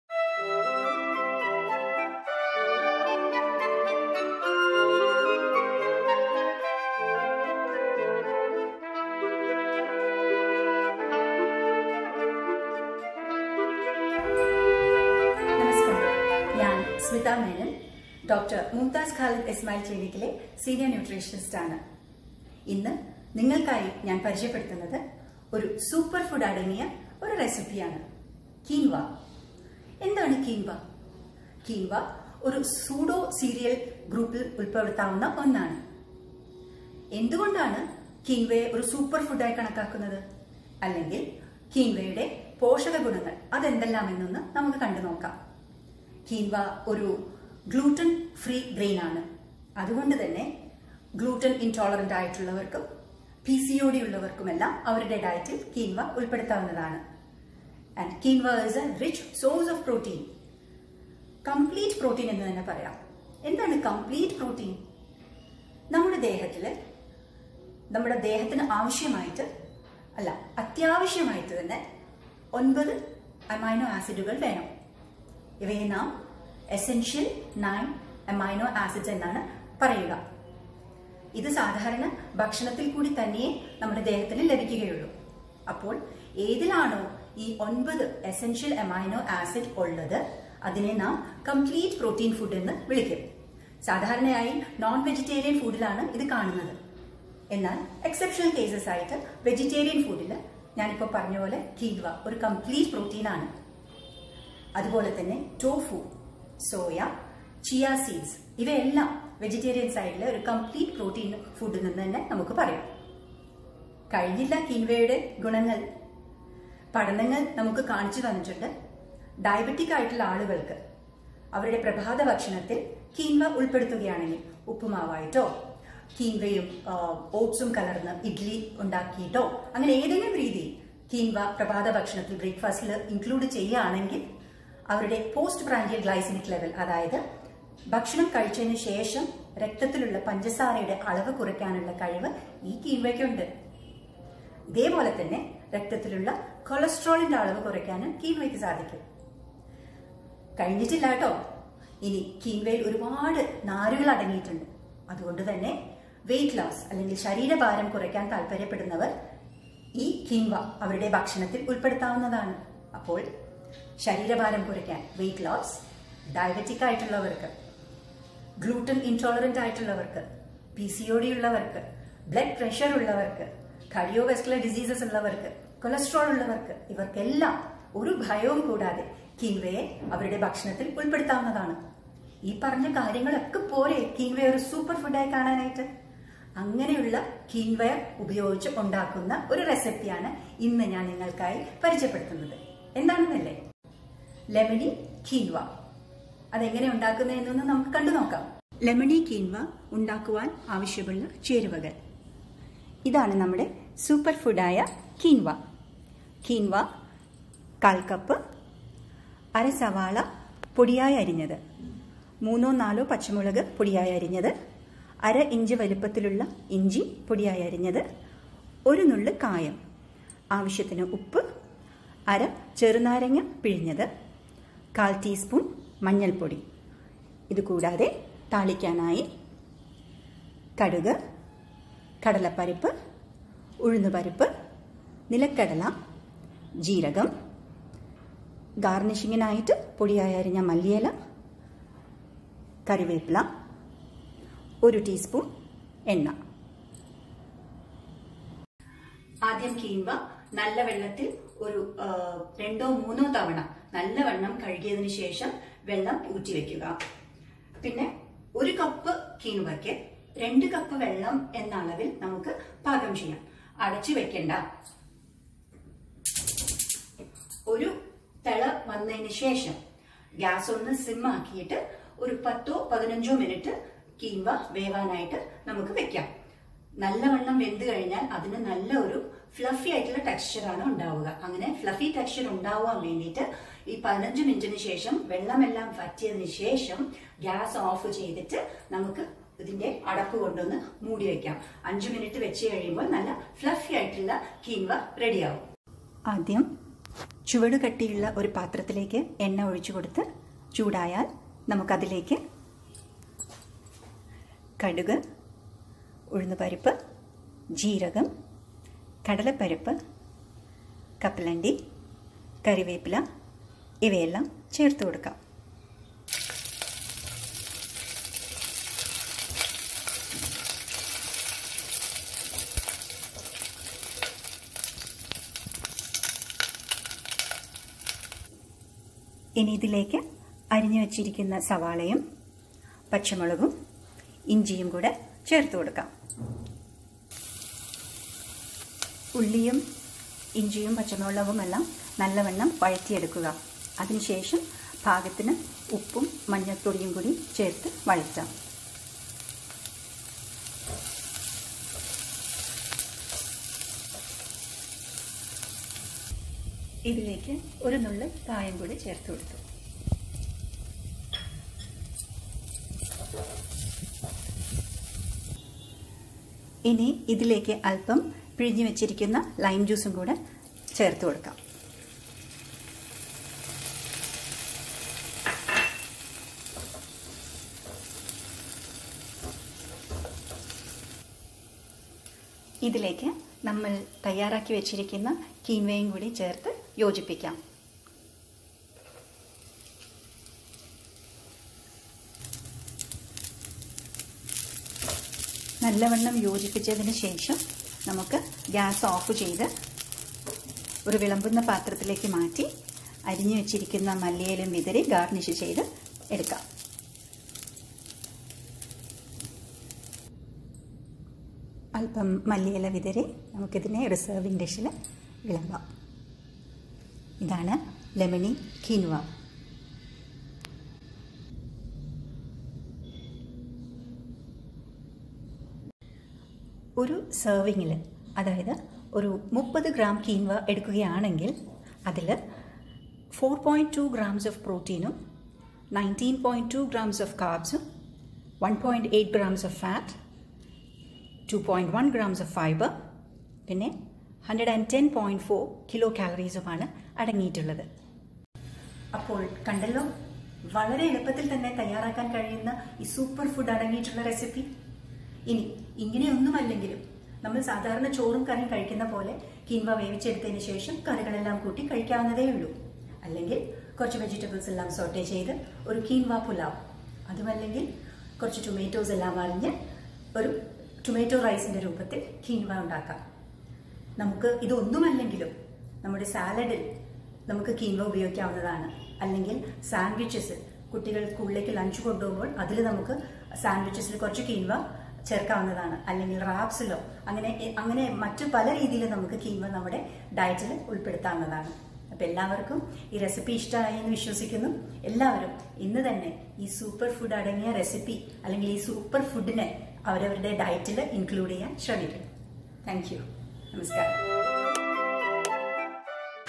സൂപ്പർ രുചിയിൽ ലെമണി കീൻവ
കീൻവ  പ്രോട്ടീനിന്റെ മികച്ച ഉറവിടമാണ്. ഒൻപത്  എസൻഷ്യൽ അമിനോ ആസിഡുകൾ അടങ്ങിയിട്ടുള്ളതിനാൽ കീൻവ  ഒരു കംപ്ലീറ്റ് പ്രോട്ടീൻ ഫൂഡ് ആണ്. പ്രമേഹരോഗികൾ അവരുടെ പ്രഭാതഭക്ഷണത്തിൽ കീൻവ  ഉൾപ്പെടുത്തുന്നതിലൂടെ,  ഭക്ഷണം കഴിച്ചശേഷം രക്തത്തിലുള്ള പഞ്ചസാരയുടെ അളവ് കുറയ്ക്കാനാകുന്നുവെന്നു പഠനങ്ങൾ പറയുന്നു.  രക്തത്തിലെ കൊളസ്ട്രോളിന്റെ അളവു കുറയ്ക്കാനും കീൻവയ്ക്കു സാധിക്കും. കീൻവയിൽ  നാരുകൾ ധാരാളമുള്ളതിനാൽ ശരീരഭാരം കുറയ്ക്കാനാഗ്രഹിക്കുന്നവർക്ക് ഇതു ധൈര്യമായി ഡയറ്റിൽ ഉൾപ്പെടുത്താം. രക്താതിസമ്മർദമുള്ളവർക്കും കാർഡിയോവാസ്കുലാർ രോഗങ്ങളുള്ളവർക്കും കീൻവ ഏറെ ഗുണം ചെയ്യും.